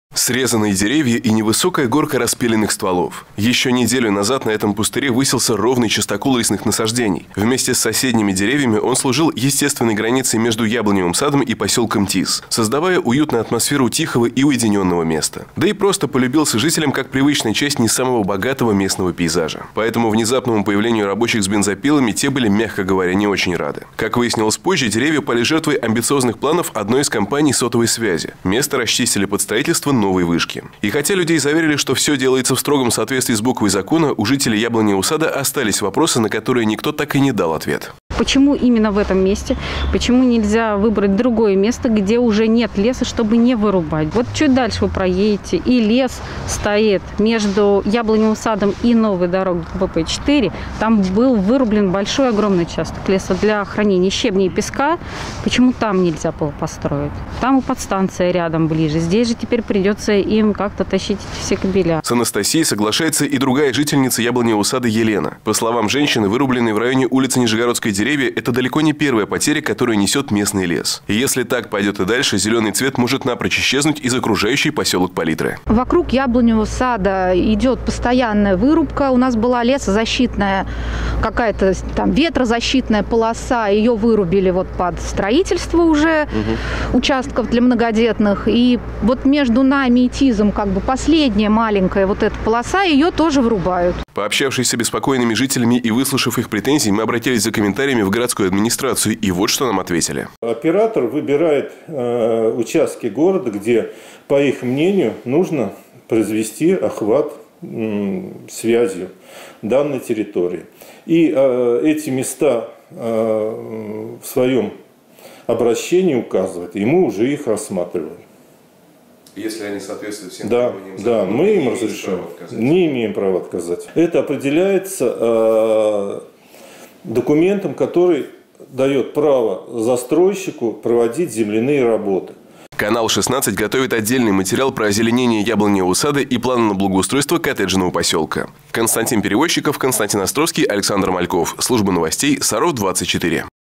Вместо деревьев - вышка сотовой связи
Жители Яблоневого сада недовольны тем, что под их окнами вырубают лес и планируют строить вышку сотовой связи. Горожане уверены, для неё есть более подходящие места. Почему объект решили поставить именно там и есть ли шанс что-то изменить?

Телефон «Городского дозора» 37-200.